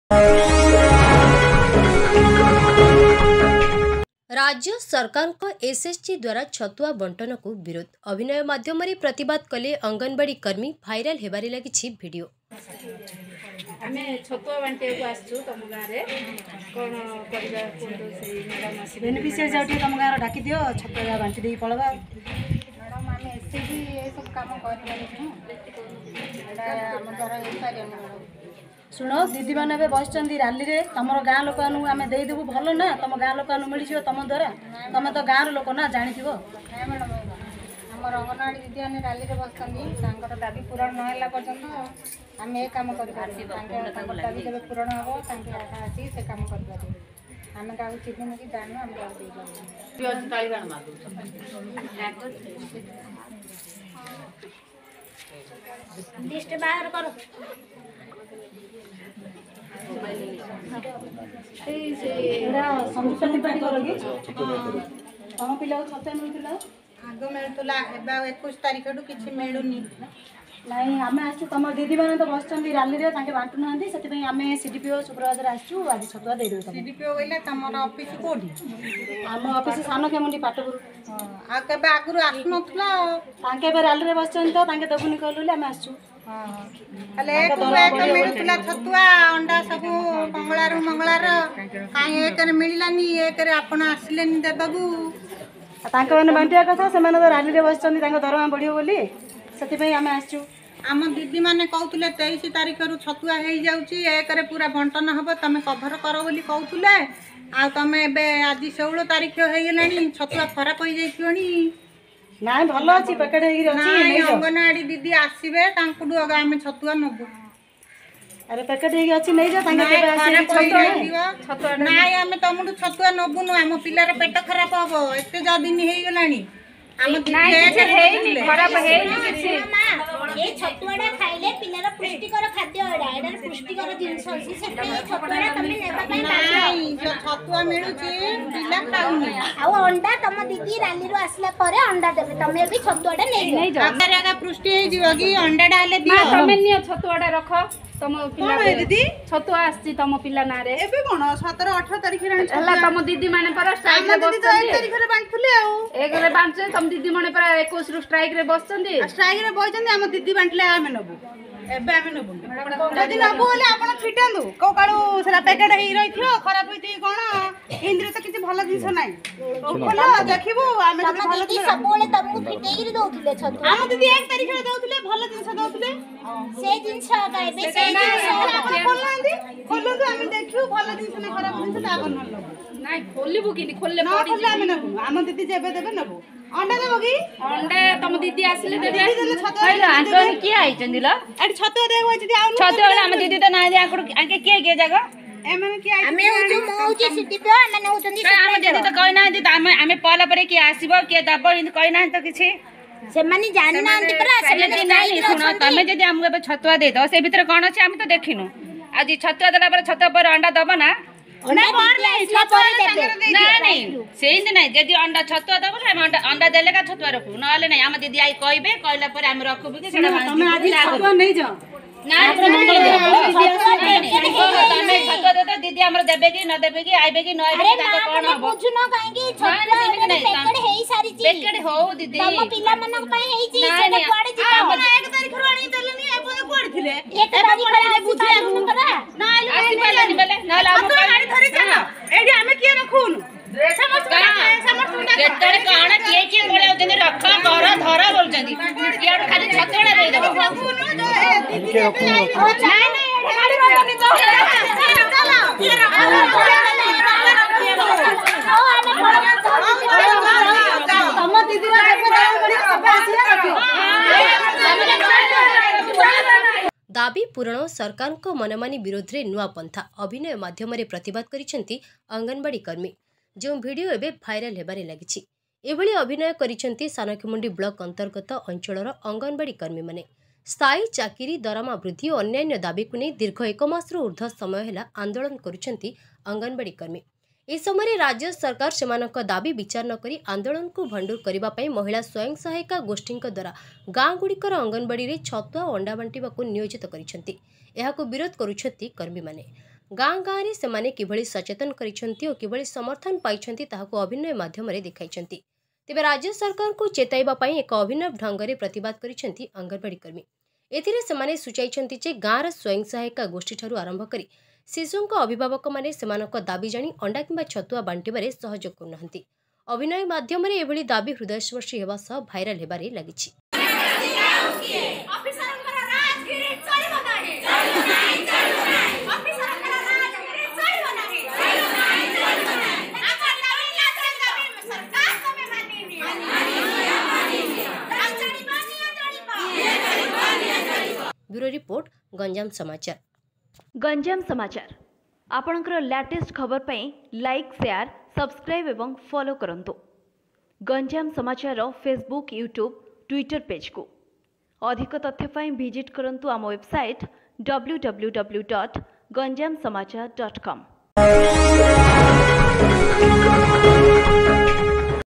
ଏସଏସଜି ମାଧ୍ୟମରେ ଛତୁଆ ବଣ୍ଟନକୁ ବିରୋଧ କରି ଅଭିନୟ ମାଧ୍ୟମରେ ସରକାରଙ୍କୁ ଦାବି ଜଣାଇଲେ ଆଶା କର୍ମୀ।
ଗଞ୍ଜାମ ସମାଚାର//୧୬//୧୨//୨୦୨୨// ଏସଏସଜି ମାଧ୍ୟମରେ ଛତୁଆ ବଣ୍ଟନକୁ ବିରୋଧ କରି ଅଭିନୟ ମାଧ୍ୟମରେ ସରକାରଙ୍କୁ ଦାବି ଜଣାଇଲେ ଆଶା କର୍ମୀ।